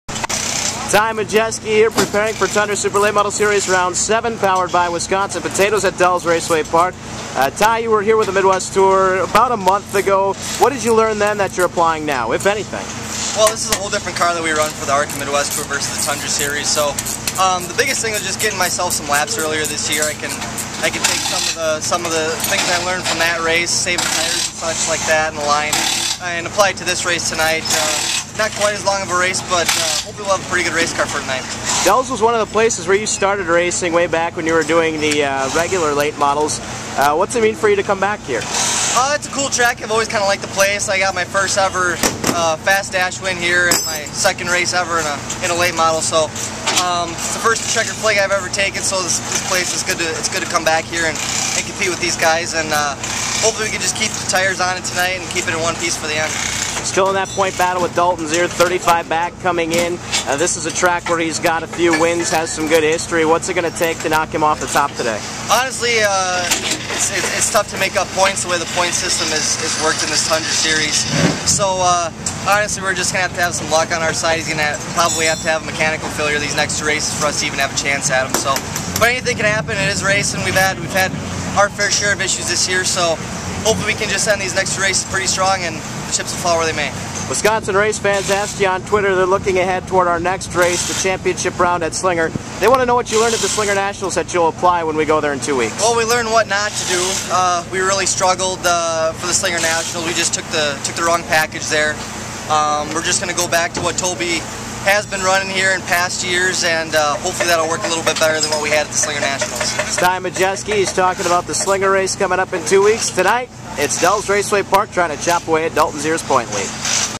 Ty Majeski TUNDRA Round Seven Pre-Race
Ty Majeski discusses TUNDRA Round Seven at Dells Raceway Park before the events on 8-23-14.